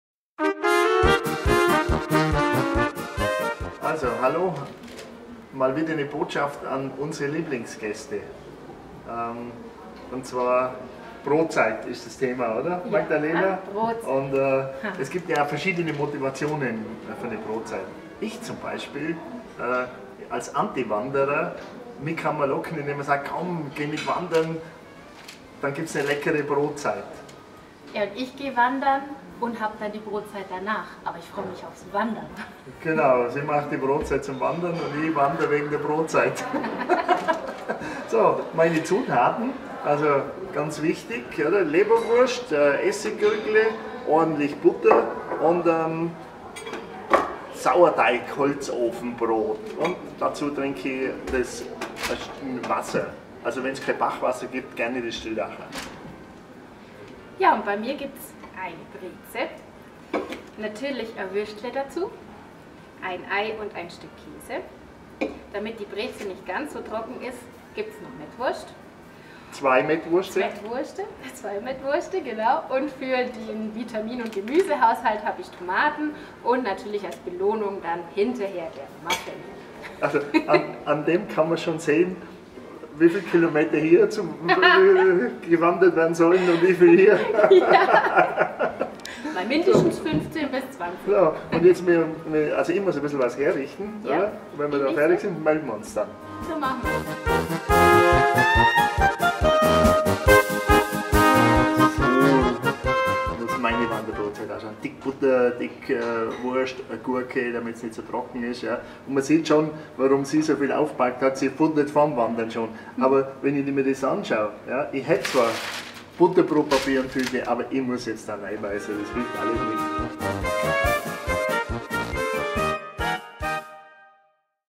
Brotzeit im Hotel Oberstdorf
Wer die Wahl hat, hat die Qual! Für Ihre Wanderung in den Oberstdorfer Bergen braucht es eine ordentliche Brotzeit für unterwegs. Oder doch nicht? Magdalena und Sebastian haben da recht unterschiedliche Herangehensweisen.